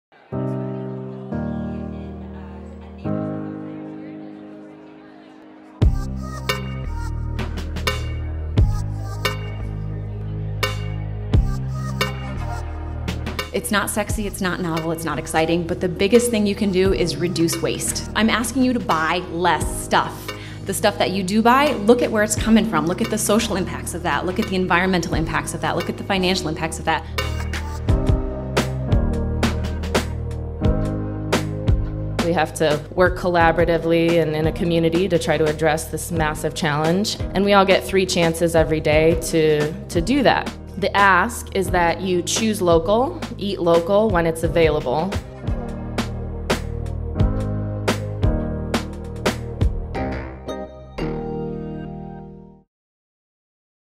IC Climate Expo - Recap #2
The City of Iowa City held our first IC Climate Expo to discuss our new
Climate Action Plan goals and allow leaders from across the community to discuss how we can unite to combat climate change.